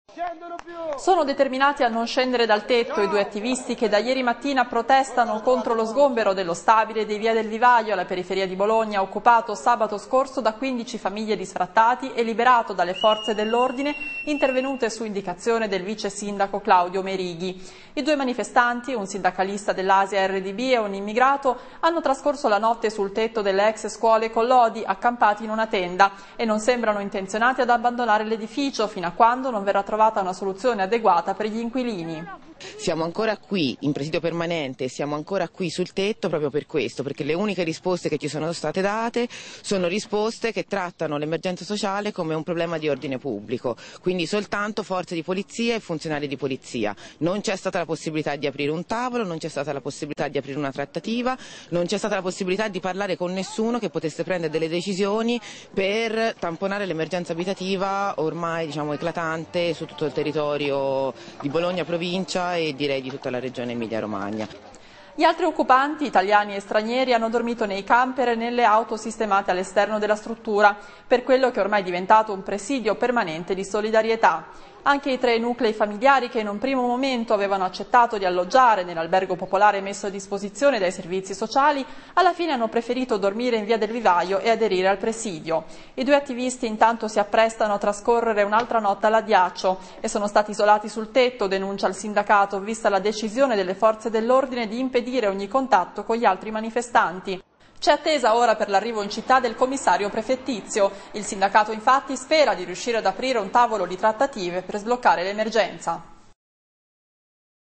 Telesanterno Edificio Occupato Bologna 17 febbraio 2010
Sono determinati a non scendere dal tetto i due attivisti che da ieri mattina protestano contro lo sgombero dello stabile di via del Vivaio, alla periferia di Bologna, occupato sabato scorso da quindici famiglie di sfrattati e liberato dalle forze dellordine, intervenute su indicazione del vicesindaco Claudio Merighi. I due manifestanti, un sindacalista dellAsia-Rdb e un immigrato hanno trascorso la notte sul tetto delle ex scuole Collodi, accampati in una tenda, e non sembrano intenzionati ad abbandonare ledificio fino a quando non verrà trovata una soluzione adeguata per gli inquilini.
Gli altri occupanti, italiani e stranieri, hanno dormito nei camper e nelle auto sistemate allesterno della struttura per quello che è ormai diventato un presidio permanente di solidarietà. Anche i tre nuclei familiari che in un primo momento avevano accettato di alloggiare nellalbergo popolare messo a disposizione dai servizi sociali, alla fine hanno preferito dormire in via del Vivaio e aderire al presidio. I due attivisti intanto si apprestano a trascorrere unaltra notte alladdiaccio e sono stati isolati sul tetto  denuncia il sindacato  vista la decisione delle forze dellordine di impedire ogni contatto con gli altri manifestanti.
Cè attesa ora per larrivo in città del commissario prefettizio: il sindacato infatti spera di riuscire ad aprire un tavolo di trattative per sbloccare lemergenza e trovare soluzioni concrete ad un problema che sta investendo una fascia sempre più ampia della popolazione.